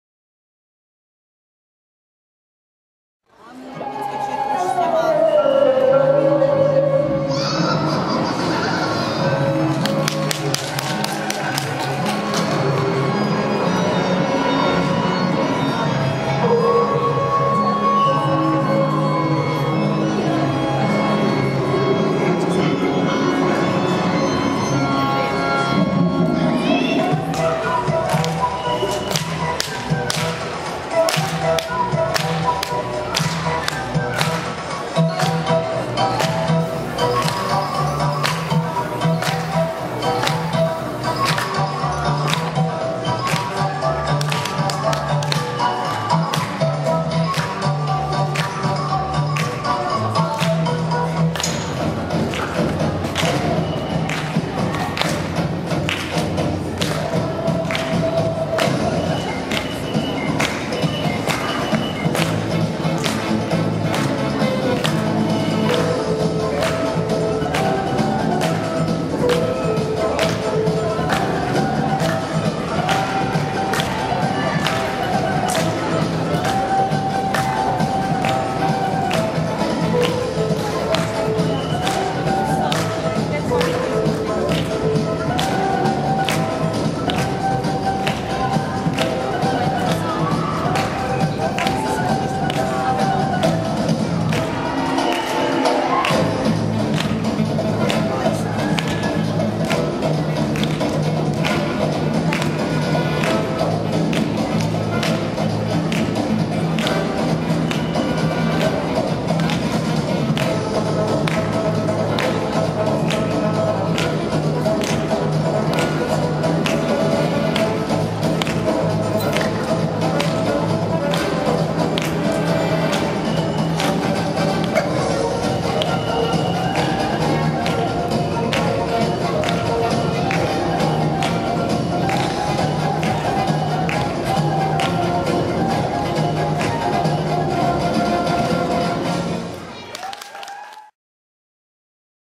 SS 0. kategorie (1), SC 80 Chomutov, 2013
Oblastní přebor společných skladeb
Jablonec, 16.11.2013
kompozice 1,75   provedení 6,00   celkem 7,75 bodu
společná skladba ve složení Sofie Krsteva, Sára Srnková, Ella Rollová, Kristýna Pirichová, Barbora Suchardová, Aneta Hajná